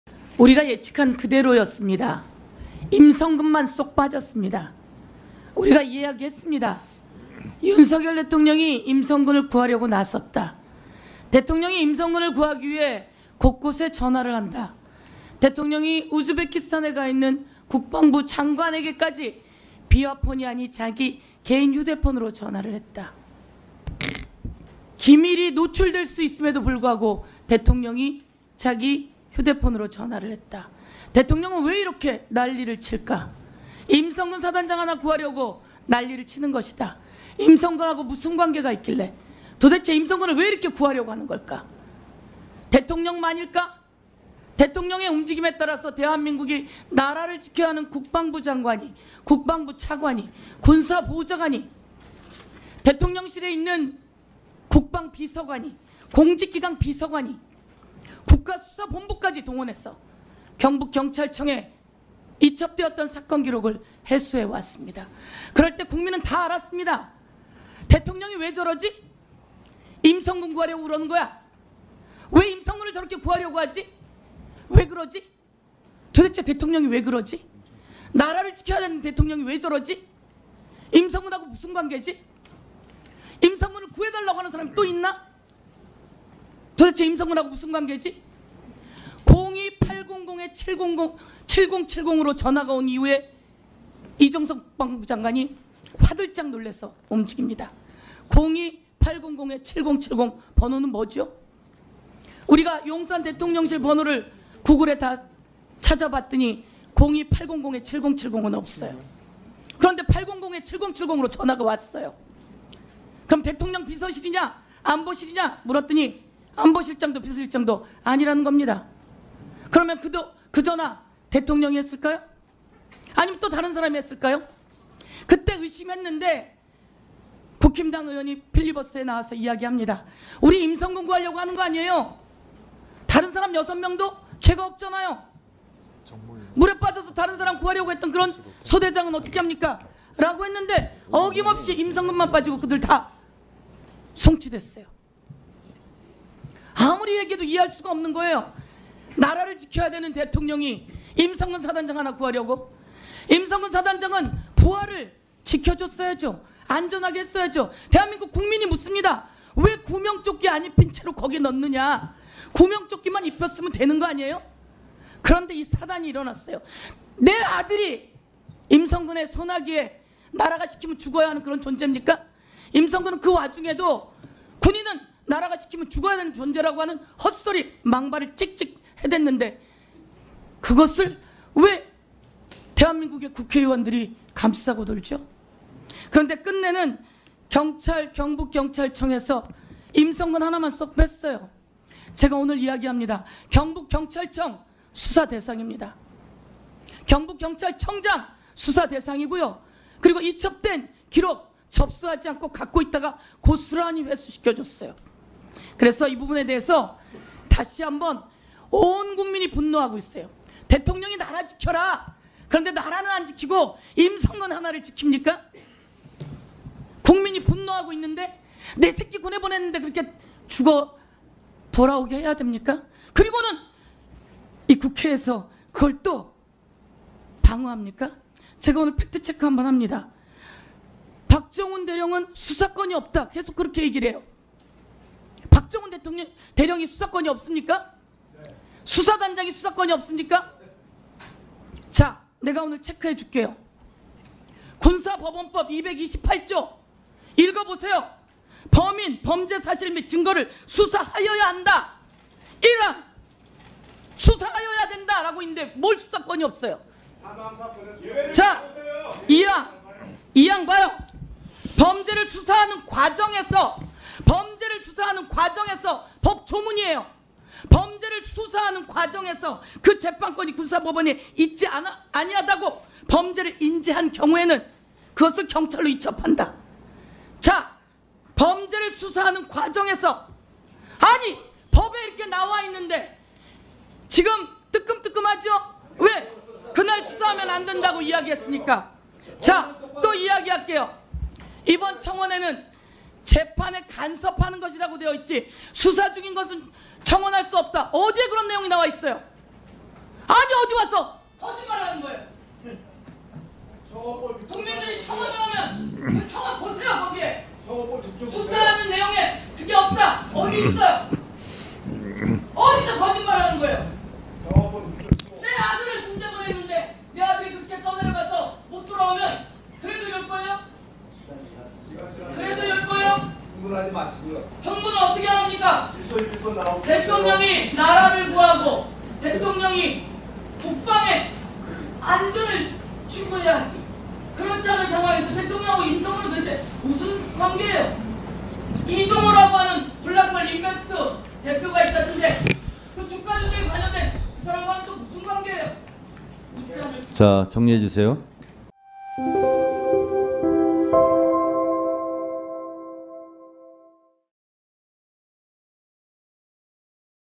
팩트 들고 앞에 앉은 유상범 박살내버린 서영교 "사실 보니 뜨끔 뜨끔 하죠?"
2024년 7월 9일 국회에서
법제사법위원회 전체회의가 진행되었다.